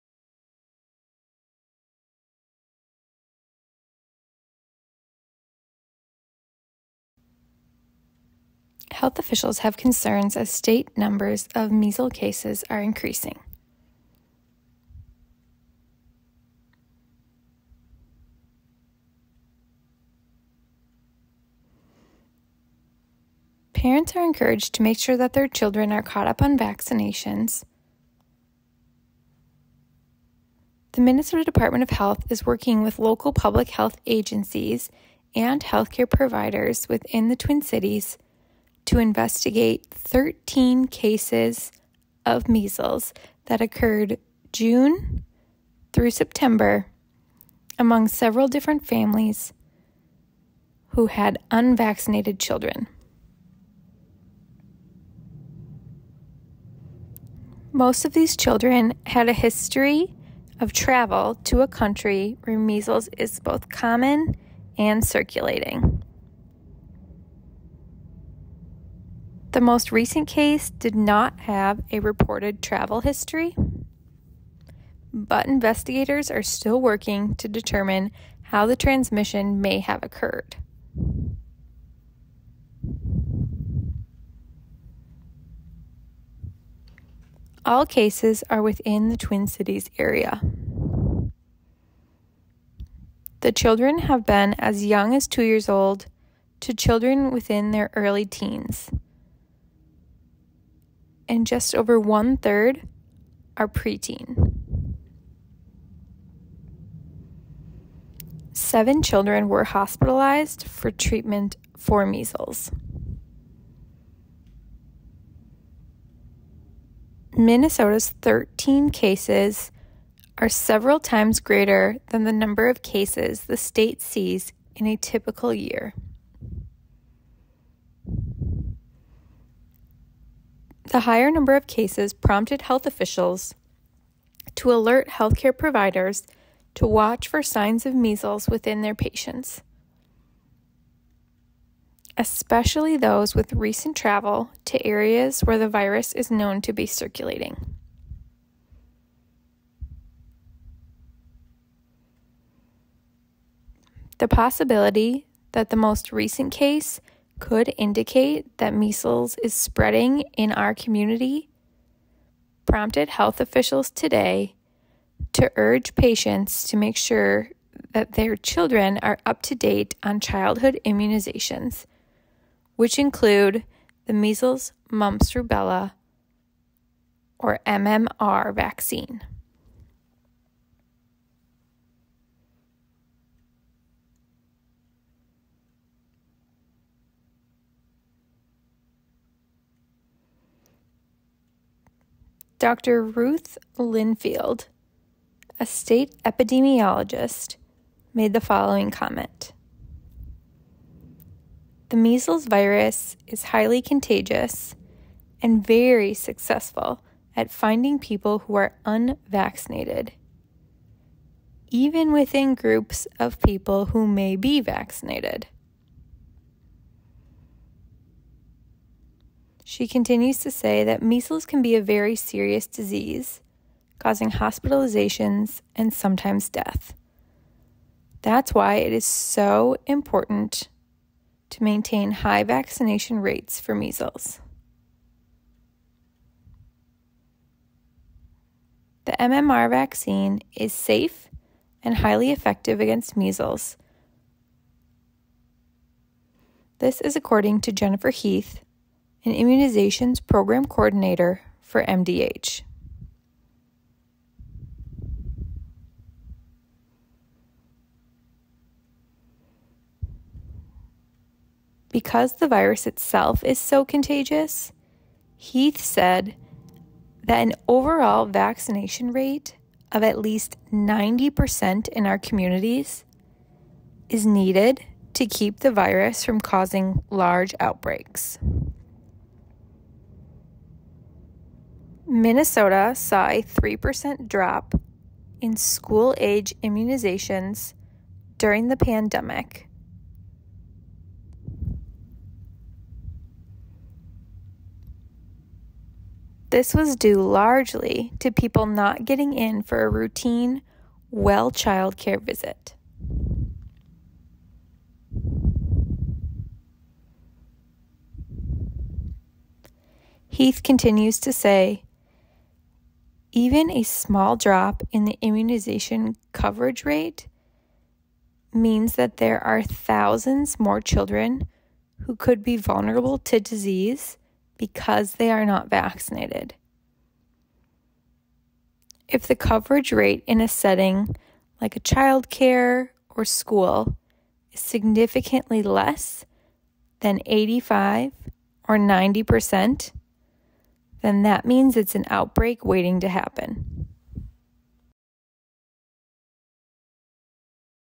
Measles Concerns Part 1
Transcript:
Health officials concerned by increase in measles cases in state, potential for outbreak
Parents urged to make sure children are up to date on immunizations
The Minnesota Department of Health (MDH) is working with local public health agencies and health care providers in the Twin Cities area to investigate 13 cases of measles that occurred June through September among several different families with unvaccinated children. Most of these children had a history of travel to a country where measles is common and circulating. The most recent case did not have a reported travel history, but investigators are still working to determine how transmission may have occurred.
All cases are in the Twin Cities area. The children have been as young as two years old to children in their early teens, and just over a third are pre-teen. Seven children were hospitalized for treatment of measles.
Minnesota’s 13 cases are several times greater than the number of cases the state sees in a typical year. The higher number of cases prompted health officials to alert health care providers to watch for signs of measles in their patients, especially those with recent travel to areas where the virus is circulating.
The possibility that the most recent case could indicate that measles is spreading in the community prompted health officials today to urge parents to make sure their children are up to date on their childhood immunizations, which include the measles-mumps-rubella (MMR) vaccine.
“The measles virus is highly contagious and very successful at finding people who are unvaccinated, even within groups of people who may be vaccinated,” said Dr. Ruth Lynfield, State Epidemiologist. “Measles can be a very serious disease, causing hospitalizations and sometimes death. That’s why it’s so important to maintain high vaccination rates for measles.”
The MMR vaccine is safe and highly effective against measles, according to Jennifer Heath, immunizations program coordinator for MDH. Because the virus itself is so contagious, Heath said, an overall vaccination rate of at least 90% in our communities is needed to keep the virus from causing large outbreaks. Minnesota saw a 3% drop in school-age immunizations during the pandemic due largely to people not getting in for routine well-child-care visits.
“Even a small drop in the immunization coverage rate means there are thousands more children who could be vulnerable to disease because they are not vaccinated,” Heath said. “If the coverage rate in a setting like a childcare or school is significantly less than 85 or 90 percent, that’s an outbreak waiting to happen,” she added.